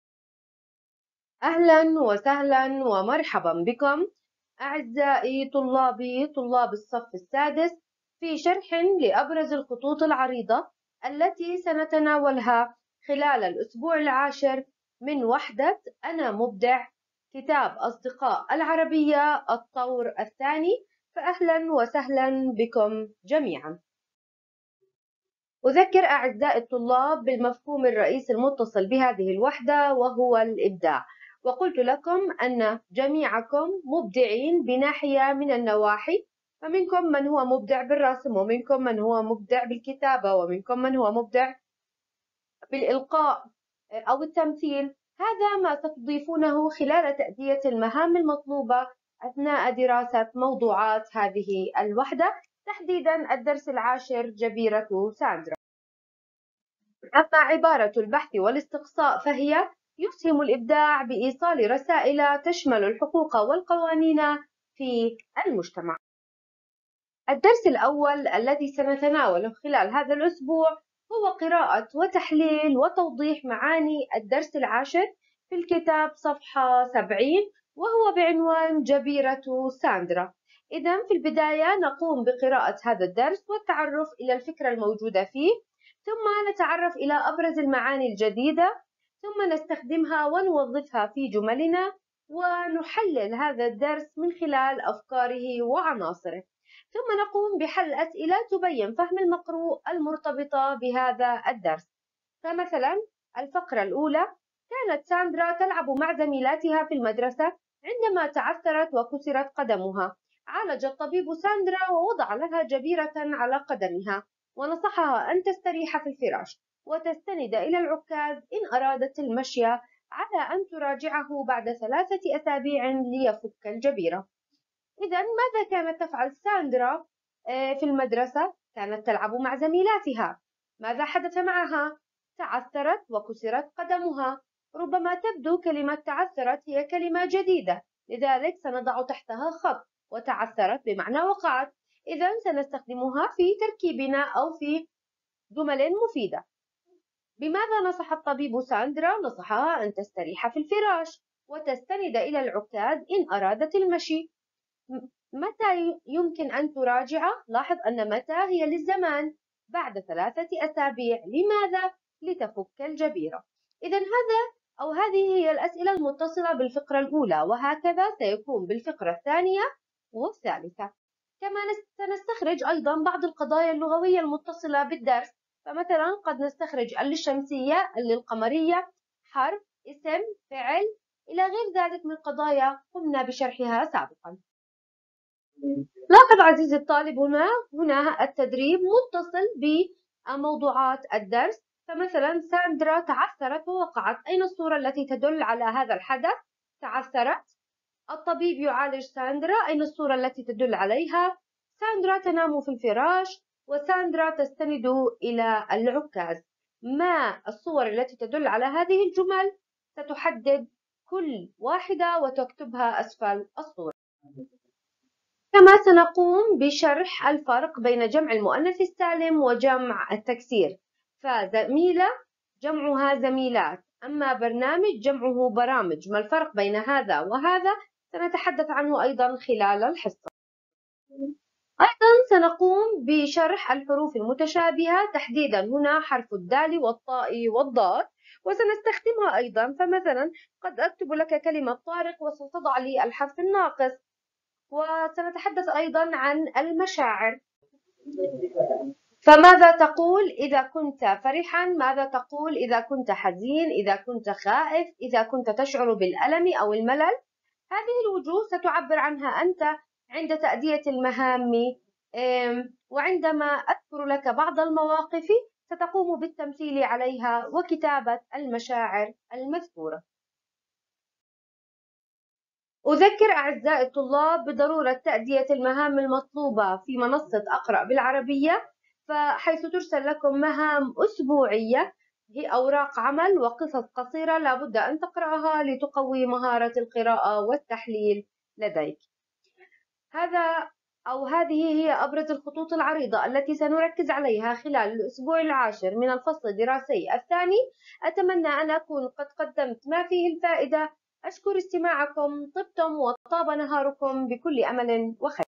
اكتساب اللغة - أنا مبدع - الطور الثاني - الأسبوع العاشر - الفصل الثاني - المعلمة بيان جابر
وصف الأسبوع العاشر

يقوم الفيديو بشرح المادّة الّتي سنقوم بشرحها خلال الأسبوع العاشر.